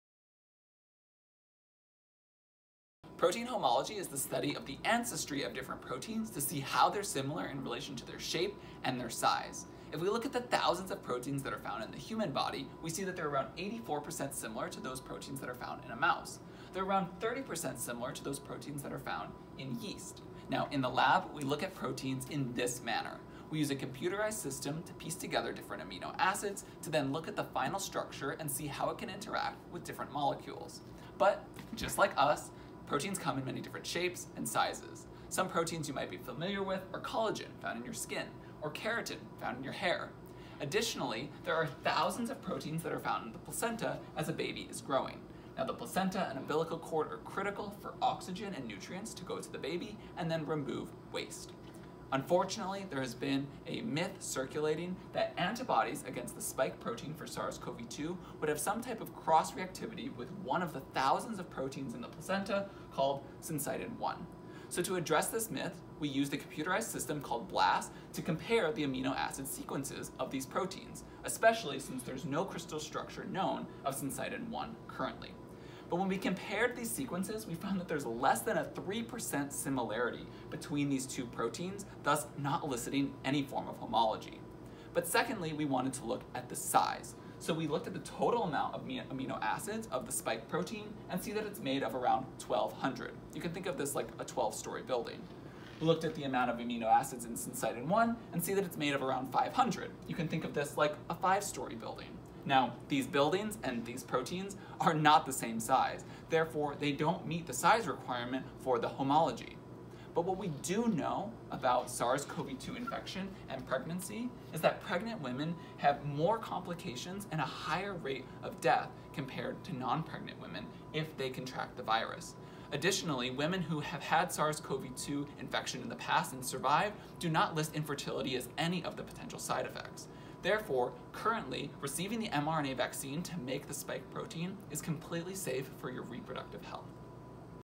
mRNA Vaccines & Pregnancy
Here I provide information on what we currently know about the spike protein of SARS-CoV-2 (produced with the mRNA vaccines) and how this relates to pregnancy. This is NOT my area of expertise, so I relied heavily on previous research & what current metrics show about vulnerable groups to Covid19.

I also received a ton of assistance from an awesome friend Ananya Anmangandla who helped to really define terms, complete the BLAST analysis, and aid me in better understanding structural comparisons! 👏ANANYA👏

Some clarifications: sequence is very important for homology but the sequence of Syncytin-1 has such low overlap with the SARS-CoV-2 protein I did not mention. The amazing chemist Ananya made this to show (use link to Twitter for picture): only the red portions on the spike protein (pictured) are similar to Syncytin-1.

Second, although a full length structure of Syncytin-1 has not been determined, partial structures are available and show very little shape similarity to the SARS-CoV-2 spike protein (again, this is the protein we will be making to elicit immunity from the mRNA vaccines).

Both proteins also come as trimers, which means that three of the same proteins come together to form this functional unit - this only further strengthens the claim that the protein sizes are vastly different.

Lastly, as of 1/13/2021 twenty three women became pregnant after participating in Pfizer's mRNA vaccine clinical trial for SARS-CoV-2, with no poor outcomes. Unfortunately, there was one poor outcome for a pregnancy in the placebo group who did not receive the vaccine.

As always, thank you for taking the time to learn more about the mRNA vaccines! Please reference my other videos for additional terminology definitions, but new technical terms are defined below.

(technical terms: Syncytin-1 = protein for placental development in humans and other primates. BLAST = [Basic Local Alignment Search Tool] a program to compare biological sequence information.)

Cover Art by: Ananya Anmangandla